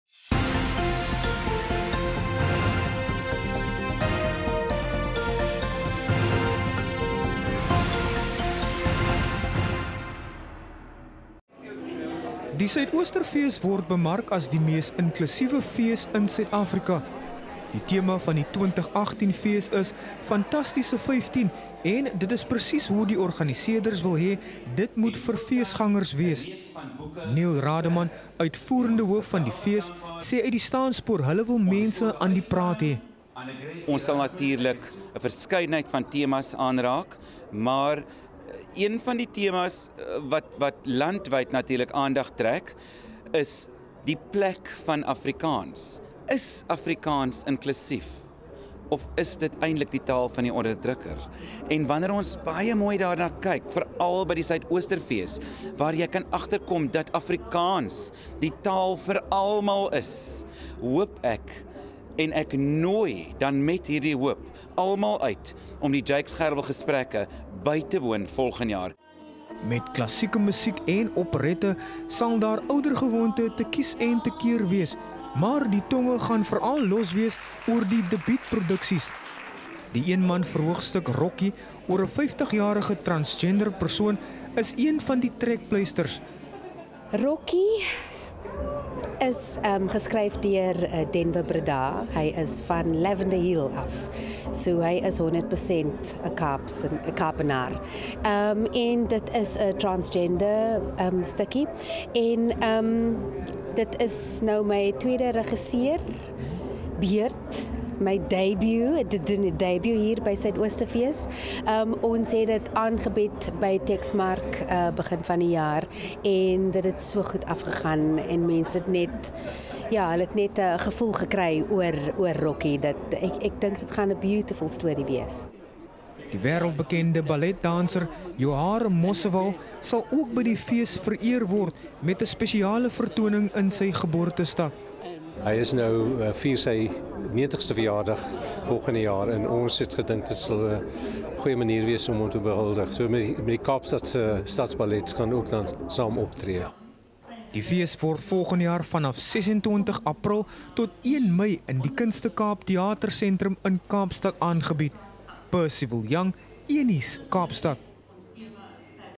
Afrikaans in kollig by Suidoosterfees
Kaapstad, 3 Desember 2017 - Die debat oor Afrikaans as taal en sy plek in Suid-Afrika word volgende jaar op die 15de Suidoosterfees weer op die spits gedryf. Die organiseerders van dié gewilde fees wil mense daarvan bewus maak dat Afrikaans 'n taal vir almal is.